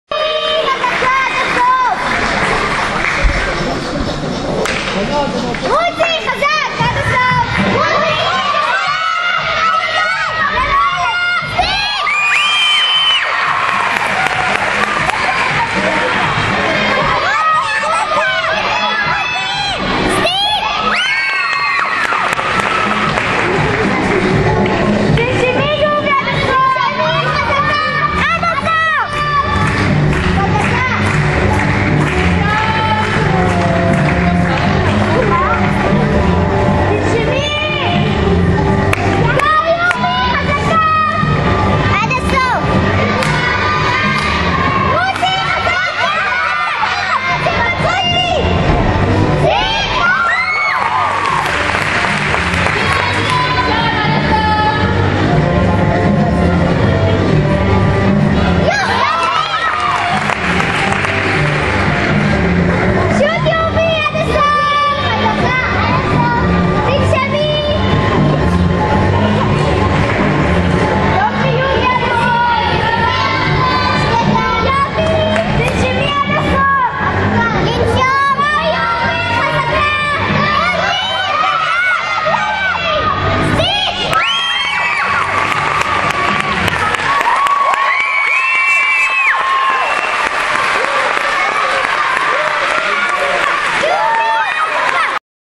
Yoval Samocha (ISR) - Floor - Maccabia 2009
Yoval Samocha (ISR) - 18th Maccabia 2009 - Junior apparatus finales Floor - 1st place - 12.925 p.